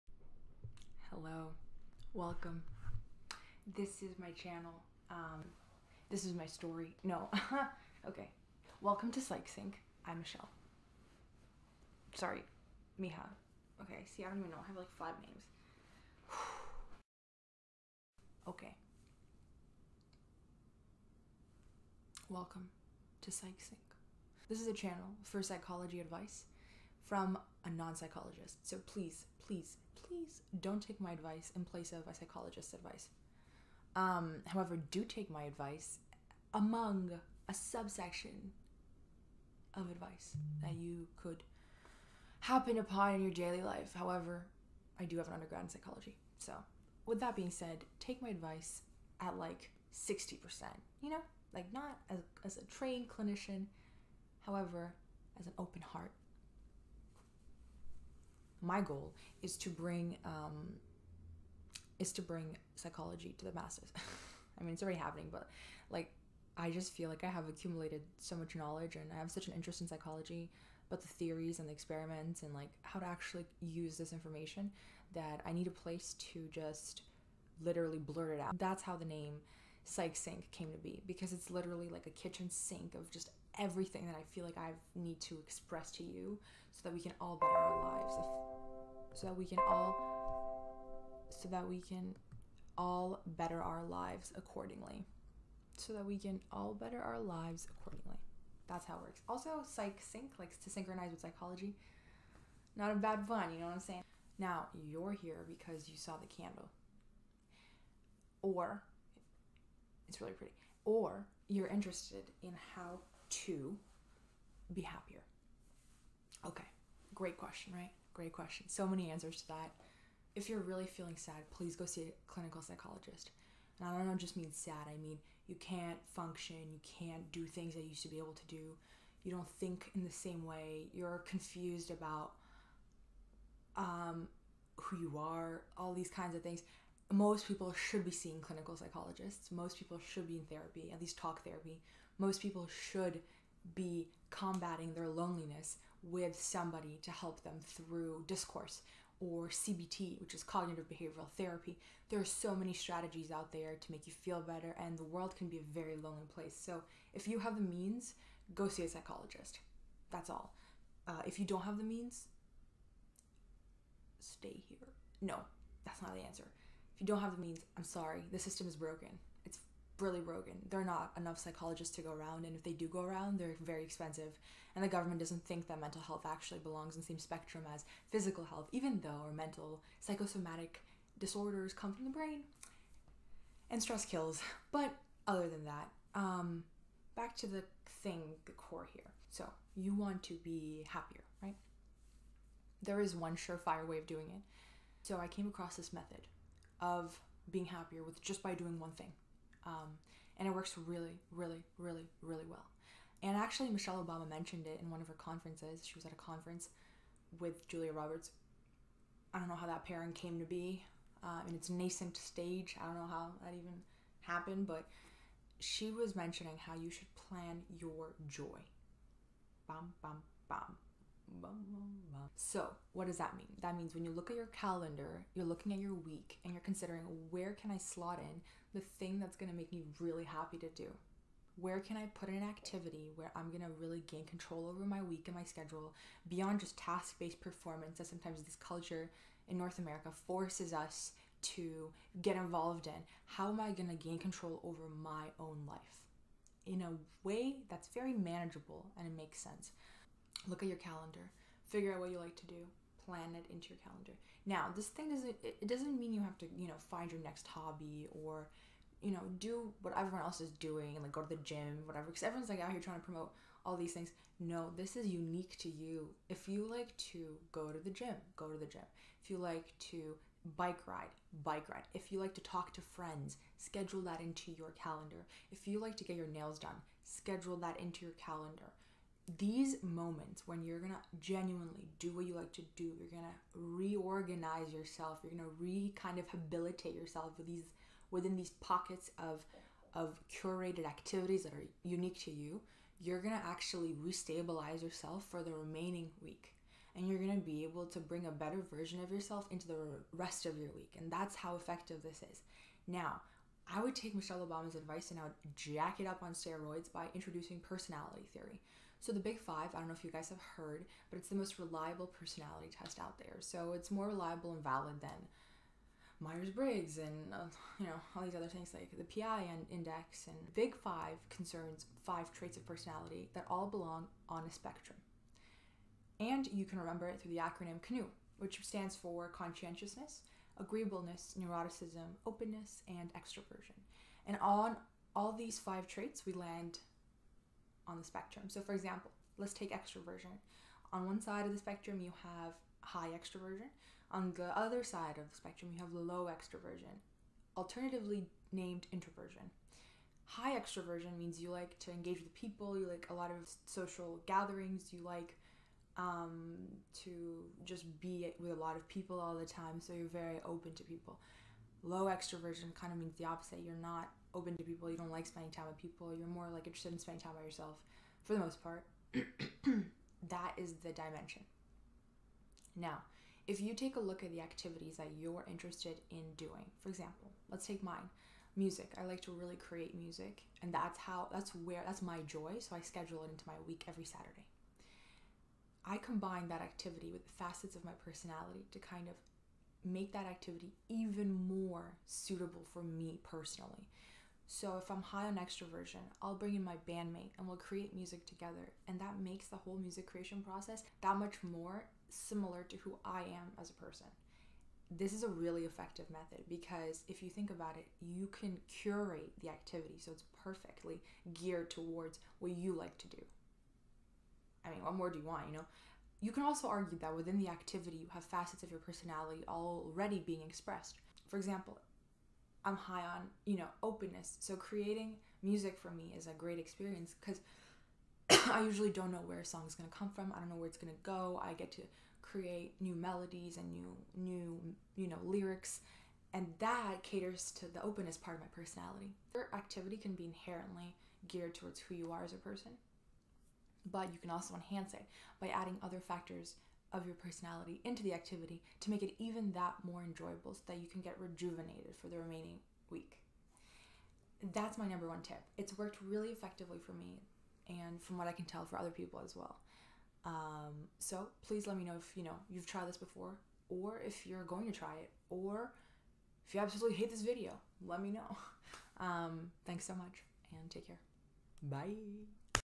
One thing that will actually, seriously make you happier
I've used this method and it actually works. It's practical, easy to do, and you can start asap. This a solution that Michelle Obama recommends, and something that I came across through my own trial and error. Please take all of my advice as entertainment. Thank you, love y'all. I appreciate the time you took out of your day to watch this. Hopefully you gained something valuable.

Skip to the actual info since I went on for yrs: 3:16